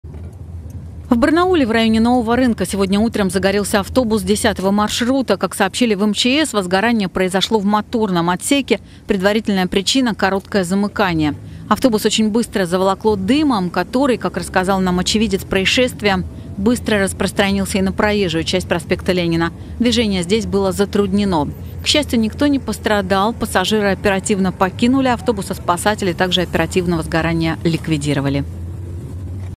В центре Барнаула загорелся автобус
В Барнауле в районе Нового рынка сегодня  утром загорелся автобус десятого маршрута. Как сообщили в МЧС, возгорание произошло в моторном отсеке, предварительная причина - короткое замыкание. Автобус очень быстро заволокло дымом, который, как рассказал  нам очевидец происшествия,   быстро распространился и на проезжую часть проспекта Ленина.   Движение здесь было затруднено. К счастью  - никто не пострадал, пассажиры оперативно покинули автобус, а спасатели также оперативно возгорание ликвидировали.